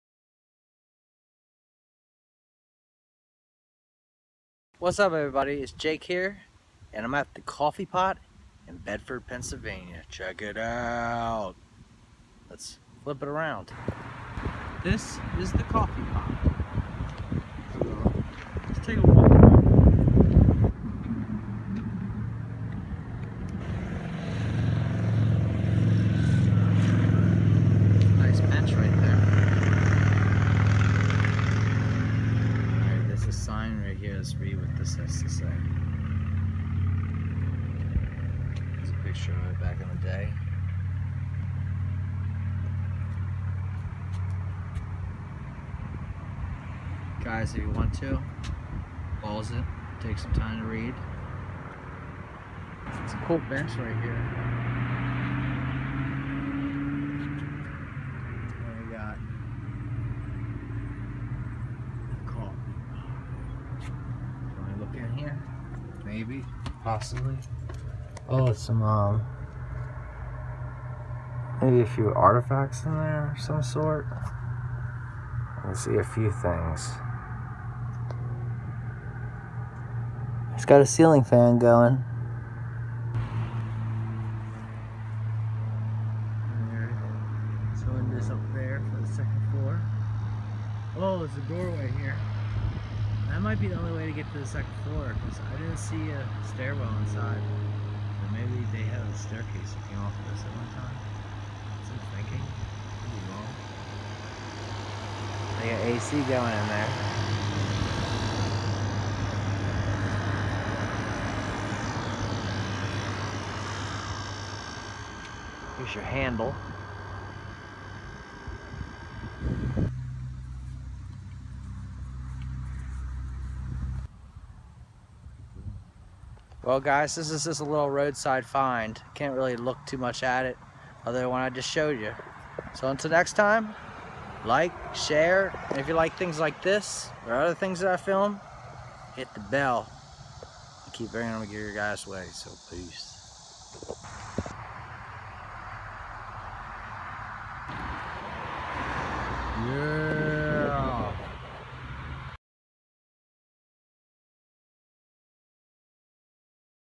The Coffee Pot! A Roadside Find! Bedford, Pennsylvania 7/26/2020
This is a short video I put together of a roadside find. Come along with me and check out this big coffee pot shaped building. Filmed 7/26/2020 in Bedford, Pennsylvania.

BTC: 3GByspGmn9Jy8UcCYWCFjpuofYek9hz9w5
ETH: 0x7DAfa5ad336CC585A9C4b75Db09922460ffE9bd8
LTC: Lc3LxuydkS4s4Yvj3BqE8BoJvigFwrHJhb